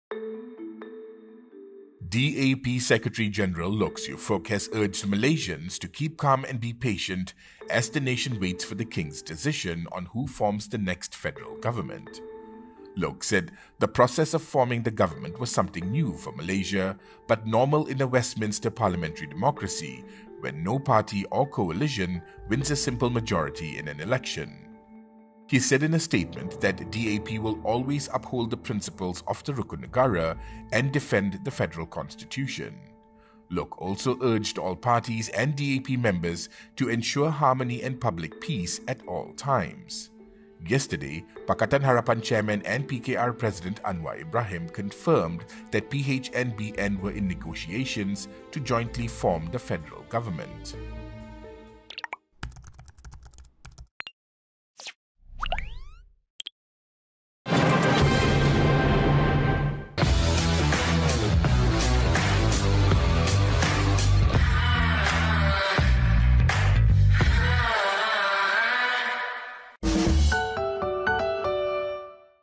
Keep calm, be patient, Loke tells Malaysians
The DAP secretary-general urges all parties to ensure harmony and public peace at all times.